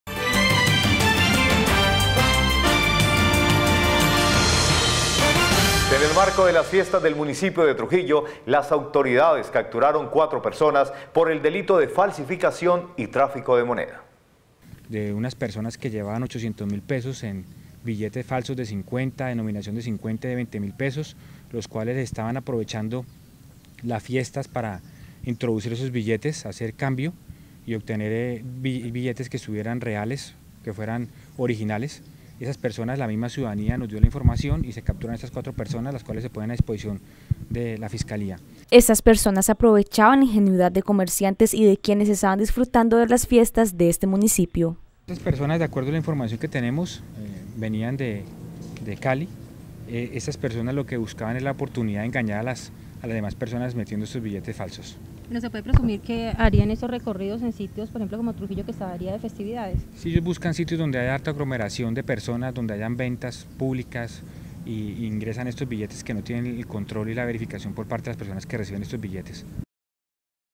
Cuatro personas capturadas por negociar con billetes falsos
En el marco de las fiestas del municipio de Trujillo, las autoridades capturaron cuatro personas por el delito de falsificación y tráfico de moneda